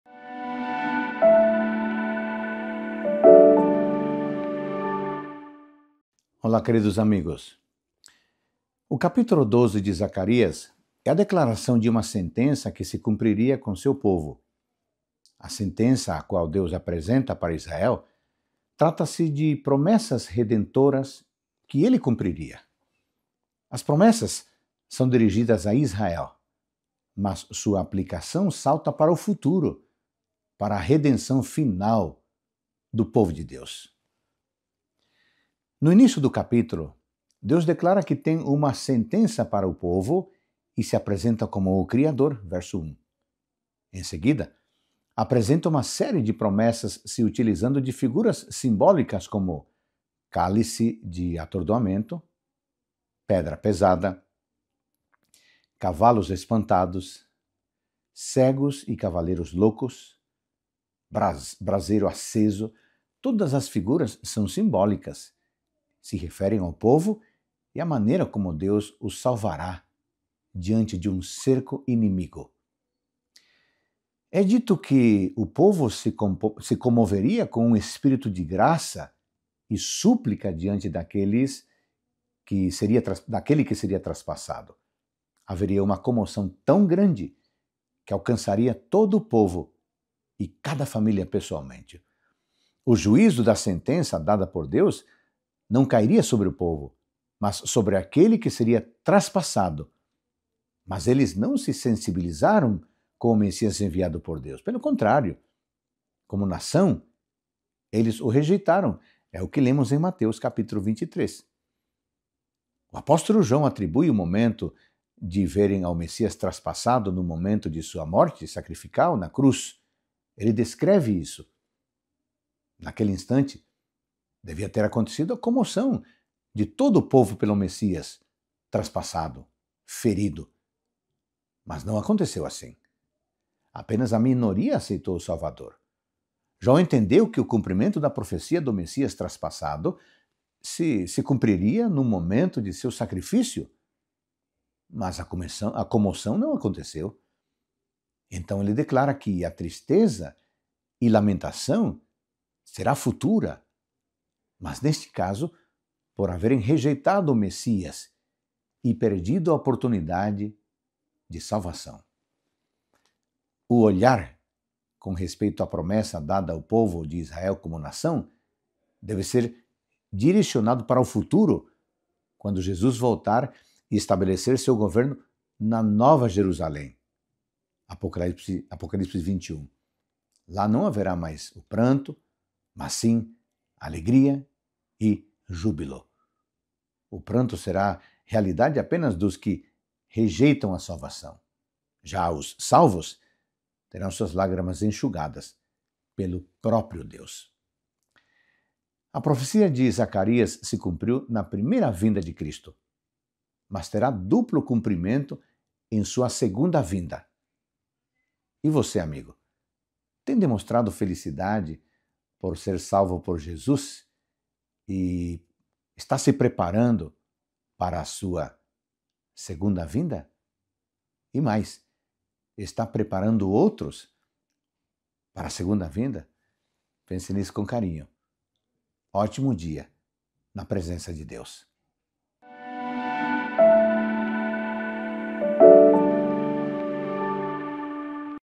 Zacarias 12 - Reavivados Por Sua Palavra
A sentença que Deus apresenta a Israel trata das promessas redentoras que ele cumpriria.